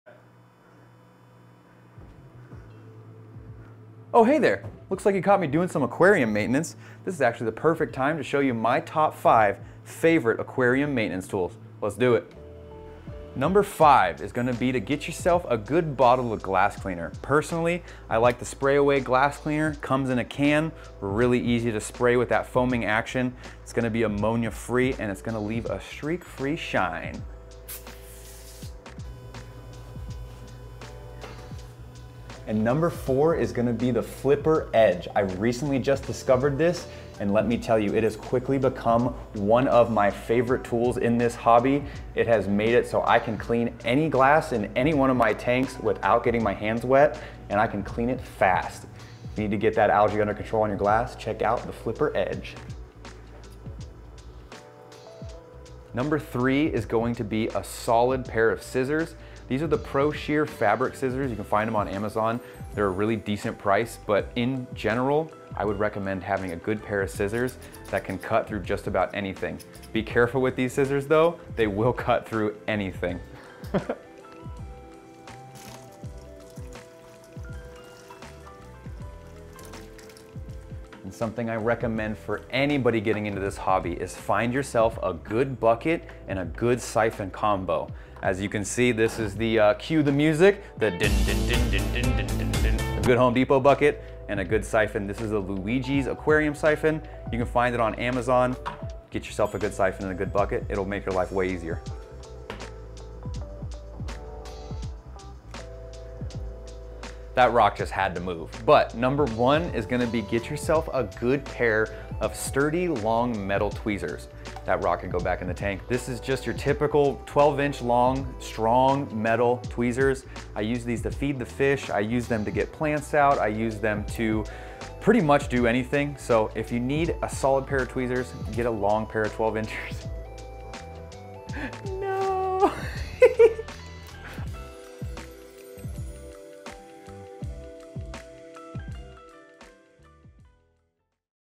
My Top 5 Favorite Aquascaping Tools!
Thought it would be fun to show you my top 5 favorite tools I use to maintain all of my tanks!

These are in order from my most used items!

Glass cleaner

Flipper Edge

ProShear Scissors

Luigis Siphon-

Bucket - (NOT THE HOME DEPOT ONE BUT STILL WORKS)